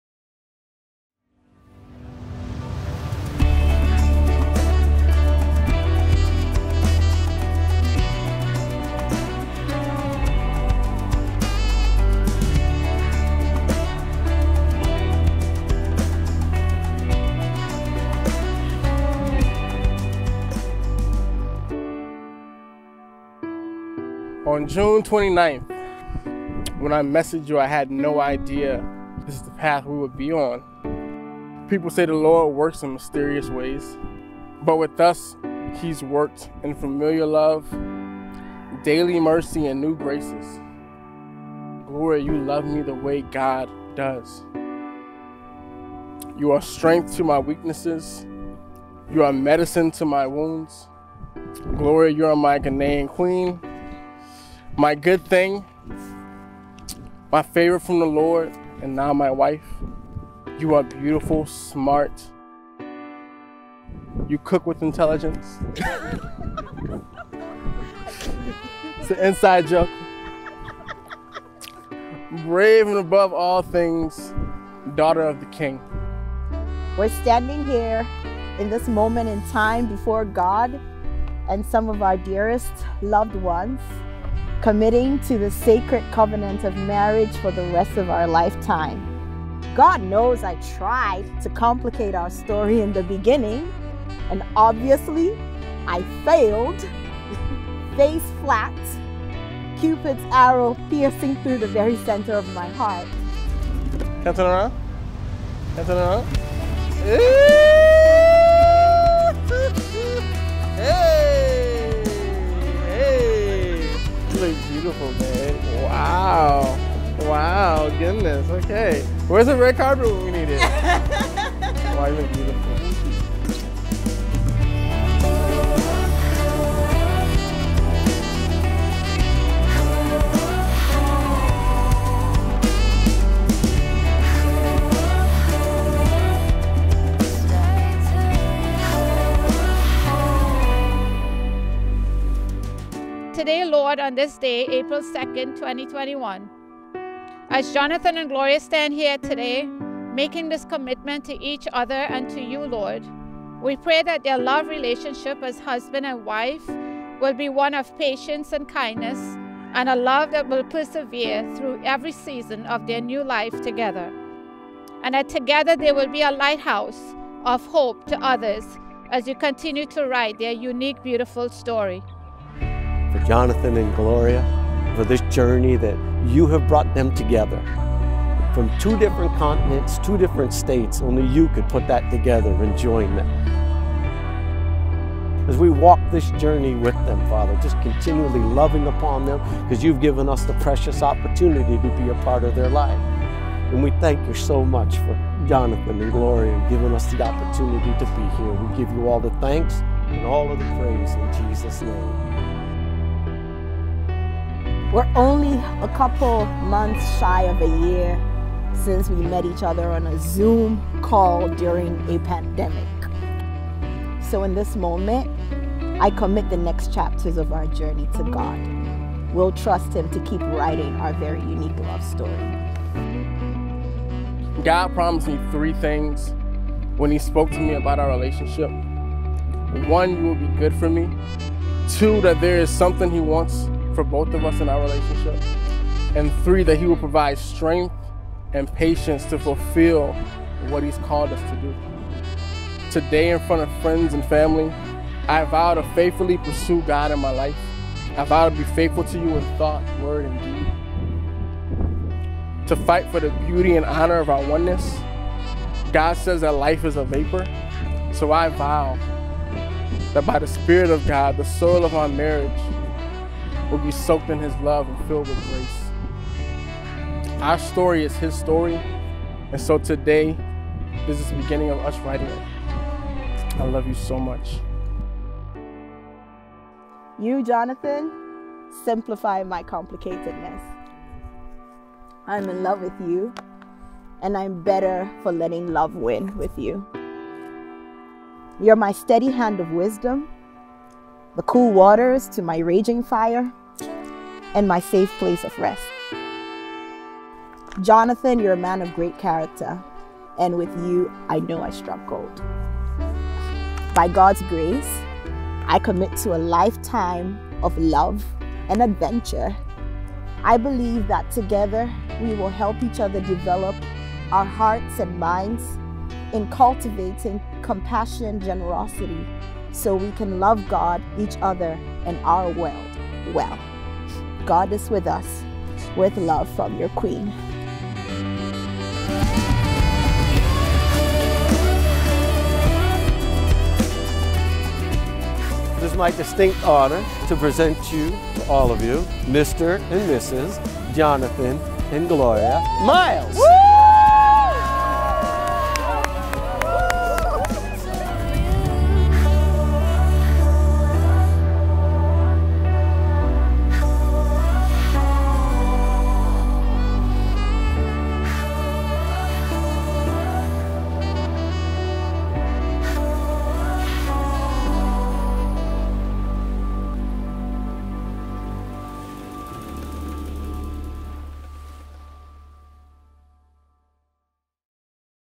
The Wedding of Gloria & Jonathan | Eastern Point Lighthouse | Gloucester, MA
Veiled In Motion Wedding Films
Boston Wedding Videographer
Rhode Island Wedding Videographer
Destination Wedding Videographer